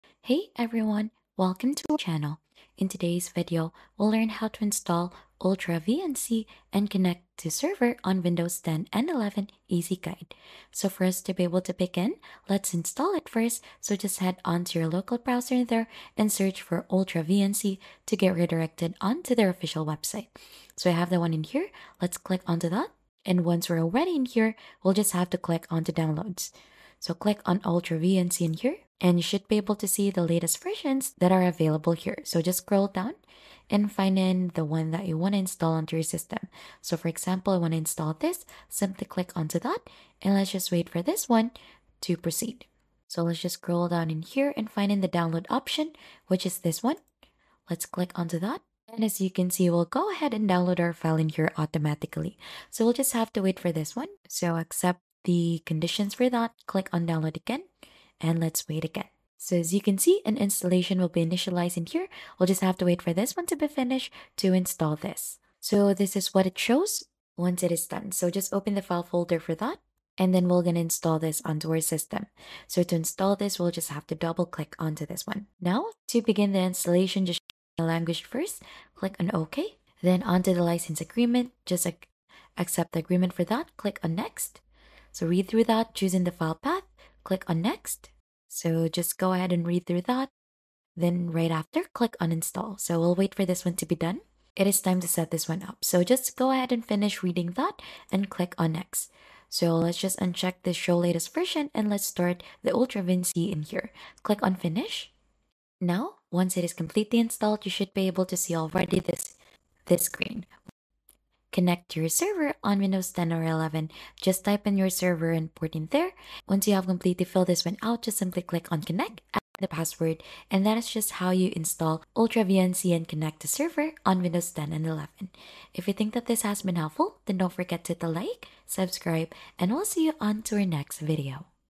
How To Install UltraVNC And Connect To Server On Windows 10 & 11 | Easy Tutorial
Set up remote desktop access effortlessly with this easy tutorial on how to install UltraVNC and connect to a server on Windows 10 and 11.

In this step-by-step guide, you’ll learn how to download and install UltraVNC, configure the server and viewer, and establish a remote connection between machines. Whether you’re managing systems, providing remote support, or accessing your own PC from another location, this tutorial makes it simple to get started with UltraVNC. All configurations are explained clearly for beginners.

Have a specific setup question or tutorial request? Drop it in the comments!